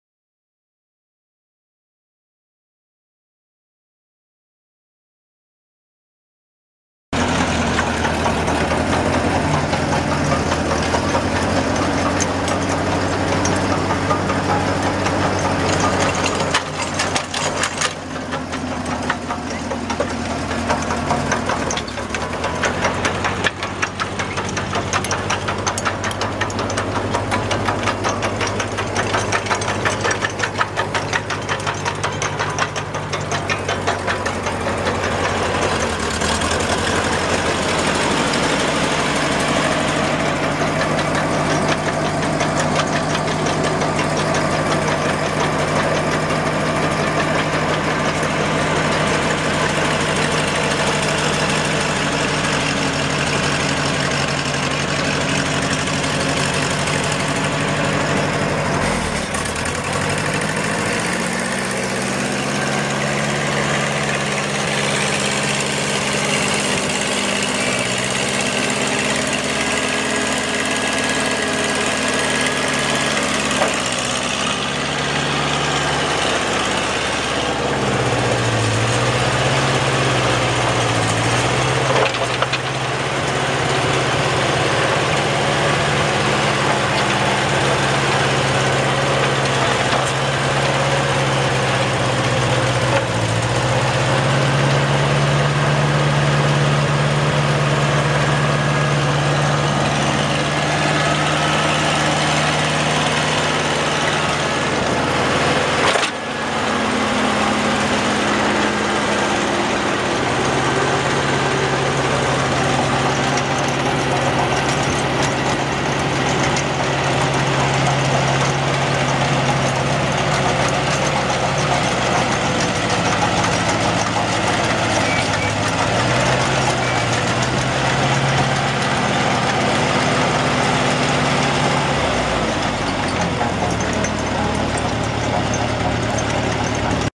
Vermeer M470 articulated trencher for sale | sold at auction July 25, 2013
Vermeer M470 articulated trencher
4,550 hours on meter
Four cylinder gas engine
Backhoe attachment
Model BH-800
18" backhoe bucket
(5) teeth
Serial 141
80"W backfill blade
6"W chain
Dirt cups
Specialty wrench
12.4-24 tires
11-24 tires
Loose fittings
Oil leaks
Operator seats damaged

It was located in Red Oak, IA, and we sold it to the highest bidder for $1,705.